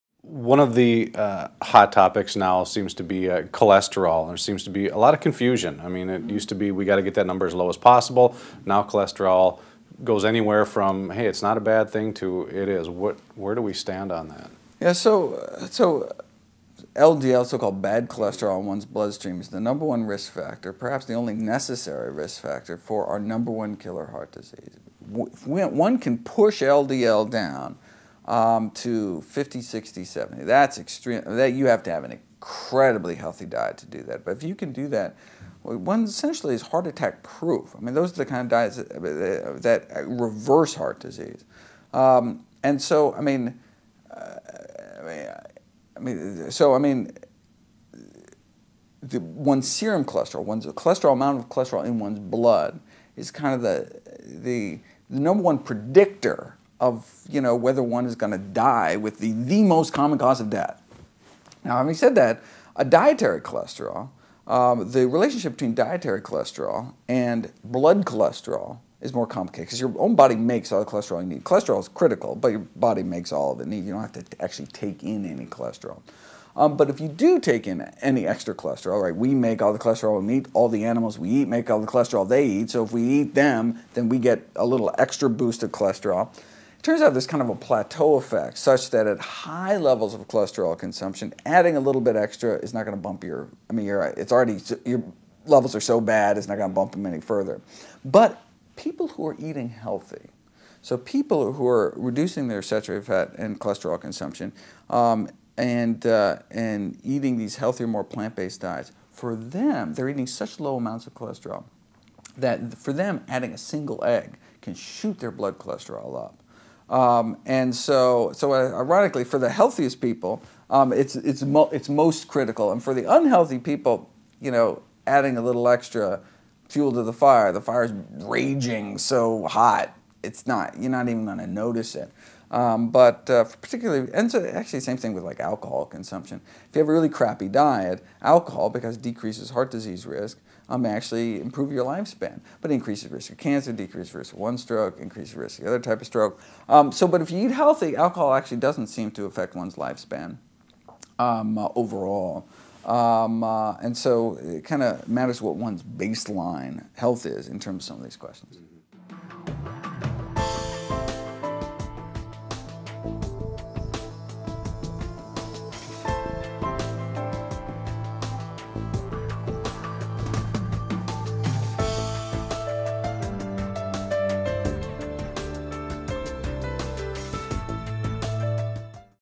Cholesterol Confusion Cleared Up!
Join tens of thousands of others and get our free email newsletter

There is still so much confusion when it comes to cholesterol.  Dr. Michael Greger clears up some of that in this video.  He discusses blood cholesterol and how your diet may have a surprising effect even  after consuming small amounts!  Find out how your diet effects future intake of cholesterol from foods!